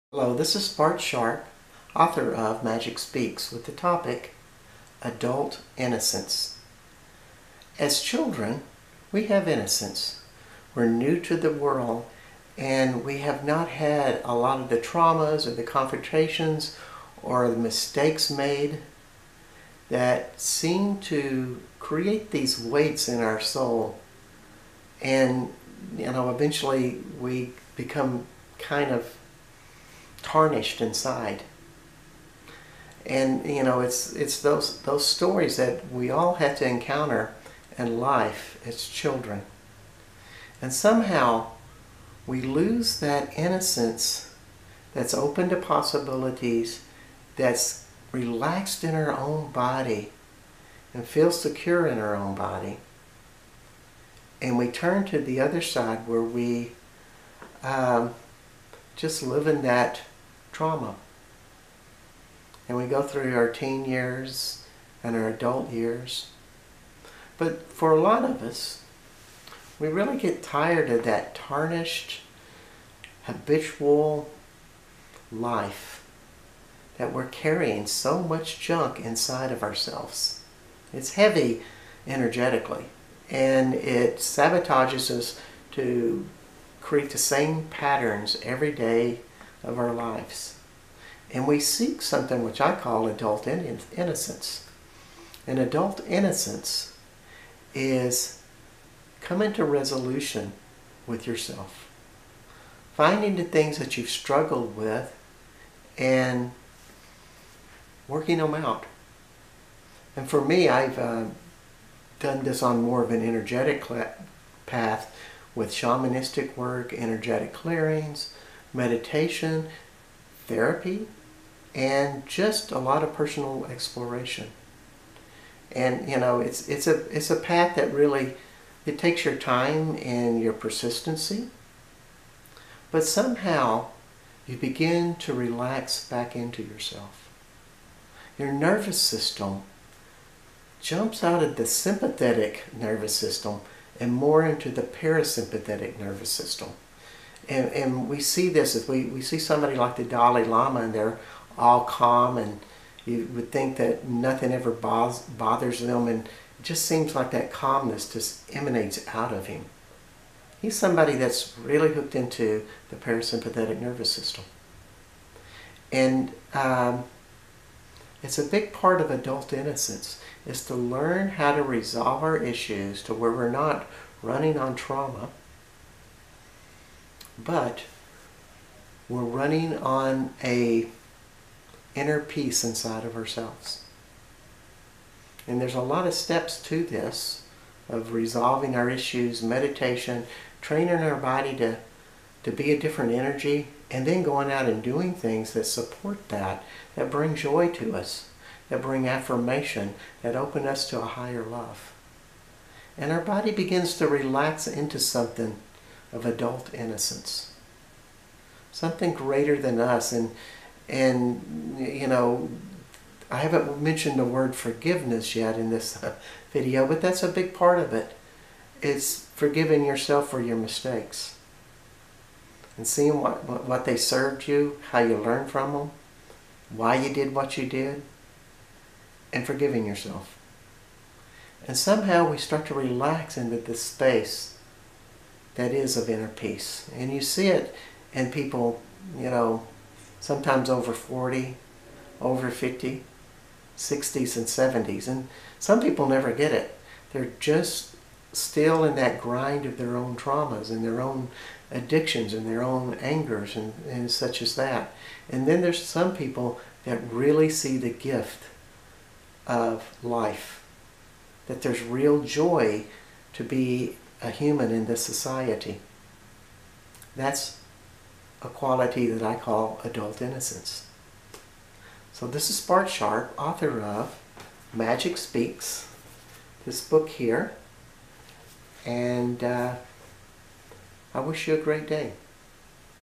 Adult Innocence
In emotional transformation we are on a journey to resolve our limitation, traumas and shadow. On the other side we mature to something more profound, adult innocence